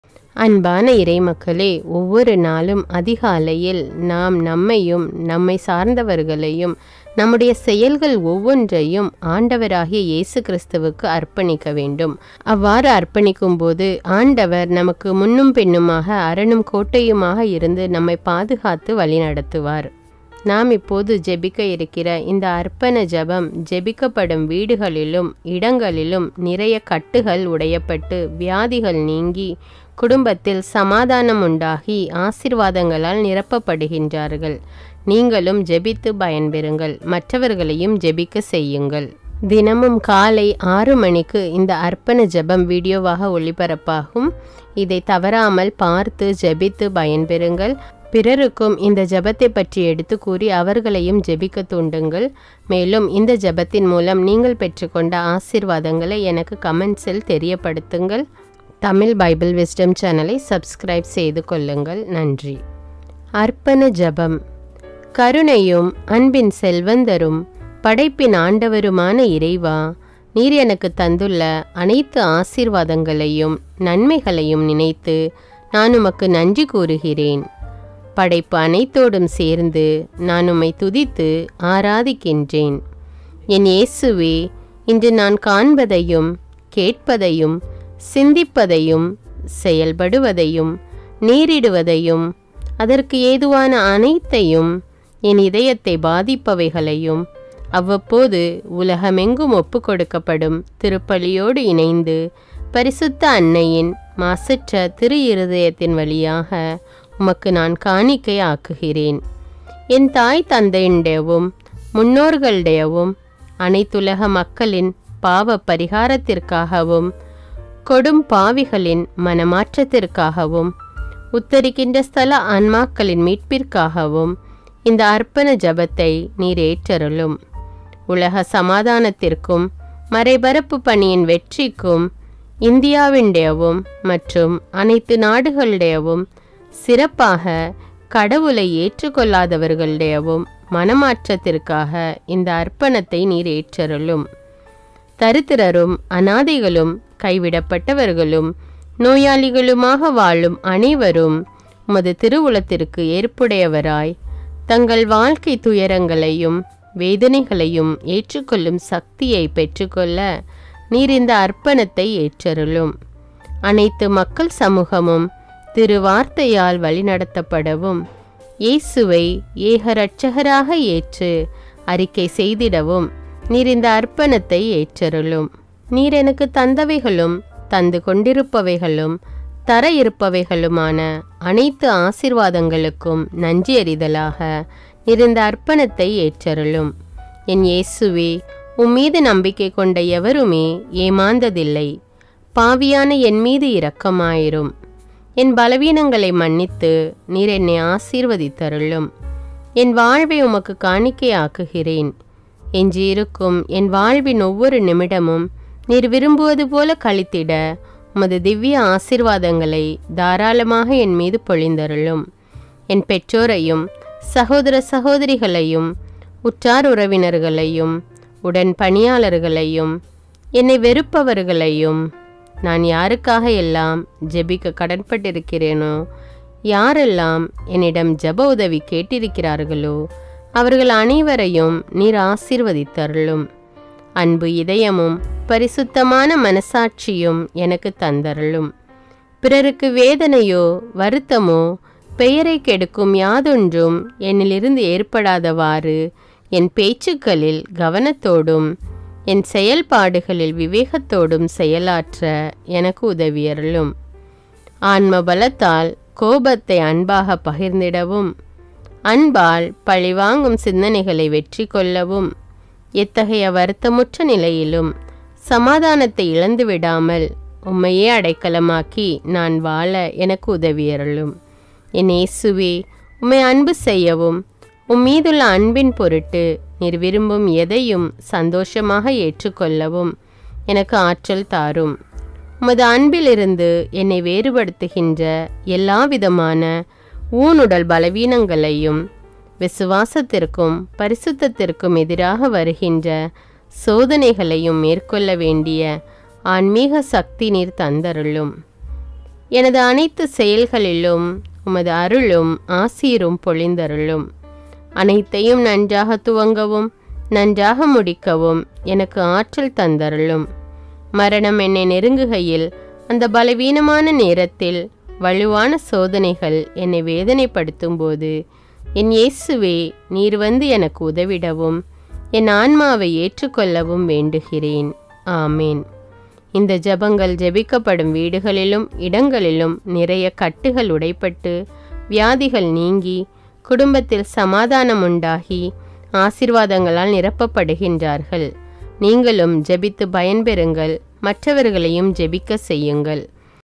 Kattugal udaiya Jebam | Prayer to break bondages in Tamil TamilBibleWisdom 6th May
அர்ப்பண ஜெபம்
கருணையும் அன்பின் செல்வந்தரும் படைப்பின் ஆண்டவருமான இறைவா! நீர் எனக்குத் தந்துள்ள அனைத்து ஆசீர்வாதங்களையும் நன்மைகளையும் நினைத்து நான்  உமக்கு நன்றி கூறுகிறேன் படைப்பு  அனைத்தோடும் சேர்ந்து நான் உம்மை துதித்து ஆராதிக்கின்றேன்.

 என் இயேசுவே! இன்று நான் காண்பதையும், கேட்பதையும், சிந்திப்பதையும், செயல்படுவதையும், நேரிடுவதையும், அதற்கு ஏதுவான அனைத்தையும், என் இதயத்தை பாதிப்பவைகளையும் அவ்வப்போது உலகமெங்கும் ஒப்புக் கொடுக்கப்படும் திருப்பலியோடு இணைத்து பரிசுத்த அன்னையின் மாசற்ற திருஇருதயத்தின் வழியாக உமக்கு நான் காணிக்கை ஆக்குகிறேன்.

 என் தாய் தந்தையினுடையவும், முன்னோர்களுடையவும், அனைத்துலக மக்களின் பாவ பரிகாரத்திற்காகவும் கொடும் பாவிகளின் மனமாற்றத்திற்காகவும், உத்தரிக்கின்ற ஸ்தல  ஆன்மாக்களின் மீட்பிற்காகவும் இந்த அர்ப்பணத்தை நீர் ஏற்றருளும். உலக சமாதானத்திற்கும், மறைப்பரப்பு பணியின் வெற்றிக்கும், இந்தியாவினுடையவும் மற்றும் அனைத்து நாடுகளுடையவும் சிறப்பாக கடவுளை ஏற்றுக் கொள்ளாதவர்களுடையவும் மனமாற்றத்திற்காக இந்த அர்ப்பணத்தை நீர் ஏற்றருளும்.
தரித்திரரும், அனாதைகளும், கைவிடப்பட்டவர்களும், நோயாளிகளுமாக வாழும் அனைவரும் உமது திருவுளத்திற்கு ஏற்புடையவராய் தங்கள் வாழ்க்கை துயரங்களையும் வேதனைகளையும் ஏற்றுக் கொள்ளும் சக்தியை பெற்றுக்கொள்ள நீர் இந்த அர்ப்பணத்தை ஏற்றருளும். அனைத்து மக்கள் சமூகமும் திரு வார்த்தையால் வழிநடத்தப்படவும் இயேசுவை ஏக இரட்சகராக ஏற்று அறிக்கை செய்திடவும் நீர் இந்த அர்ப்பணத்தை ஏற்றருளும்.
நீர் எனக்கு தந்தவைகளும், தந்து கொண்டிருப்பவைகளும், தர இருப்பவைகளுமான அனைத்து ஆசிர்வாதங்களுக்கும் நன்றியறிதலாக நீர் இந்த அர்ப்பணத்தை ஏற்றருளும். என் இயேசுவே, உம் மீது நம்பிக்கை கொண்ட எவருமே ஏமாந்ததில்லை. பாவியான என்மீது இரக்கமாயிரும். என் பலவீனங்களை மன்னித்து நீர் என்னை ஆசீர்வதித்தருளும். என் வாழ்வை உமக்கு காணிக்கையாக்குகிறேன். எஞ்சியிருக்கும் என் வாழ்வின் ஒவ்வொரு நிமிடமும் நீர் விரும்புவதுபோல கழித்திட உமது திவ்விய ஆசீர்வாதங்களை தாராளமாக என் மீது பொழிந்தருளும்.
 என் பெற்றோரையும், சகோதர சகோதரிகளையும், உற்றார் உறவினர்களையும், உடன் பணியாளர்களையும், என்னை வெறுப்பவர்களையும், நான் யாருக்காக எல்லாம் ஜெபிக்க கடன் பட்டிருக்கிறேனோ, யாரெல்லாம் என்னிடம் ஜெப உதவி கேட்டிருக்கிறார்களோ அவர்கள் அனைவரையும் நீர் ஆசீர்வதித்தருளும். அன்பு இதயமும் பரிசுத்தமான மனசாட்சியும் எனக்கு தந்தருளும்.

பிறருக்கு வேதனையோ, வருத்தமோ, பெயரை கெடுக்கும் யாதொன்றும் என்னிலிருந்து ஏற்படாதவாறு என் பேச்சுக்களில் கவனத்தோடும், என் செயல்பாடுகளில் விவேகத்தோடும் செயலாற்ற எனக்கு உதவியருளும். ஆன்ம பலத்தால் கோபத்தை அன்பாக பகிர்ந்திடவும், அன்பால் பழிவாங்கும் சிந்தனைகளை வெற்றி கொள்ளவும், எத்தகைய வருத்தமுற்ற நிலையிலும் சமாதானத்தை இழந்து விடாமல் உண்மையை அடைக்கலமாக்கி நான் வாழ எனக்கு உதவியருளும்.
என் இயேசுவே! உம்மை அன்பு செய்யவும், உம்மீதுள்ள அன்பின் பொருட்டு நீர் விரும்பும் எதையும் சந்தோஷமாக ஏற்றுக் கொள்ளவும் எனக்கு ஆற்றல் தாரும். உமது அன்பிலிருந்து என்னை வேறுபடுத்துகின்ற எல்லா விதமான உனுடல்  பலவீனங்களையும் விசுவாசத்திற்கும், பரிசுத்தத்திற்கும் எதிராக வருகின்ற சோதனைகளையும் மேற்கொள்ள வேண்டிய ஆன்மீக சக்தி நீர் தந்தருளும்.
 எனது அனைத்து செயல்களிலும் உமது அருளும் ஆசீரும் பொழிந்தருளும். அனைத்தையும் நன்றாக துவங்கவும் நன்றாக முடிக்கவும் எனக்கு ஆற்றல் தந்தருளும். மரணம் என்னை நெருங்குகையில் அந்த பலவீனமான நேரத்தில் வலுவான சோதனைகள் என்னை வேதனைப்படுத்தும் போது என் இயேசுவே நீர் வந்து எனக்கு உதவிடவும் என் ஆன்மாவை ஏற்றுக் கொள்ளவும் வேண்டுகிறேன். ஆமென்!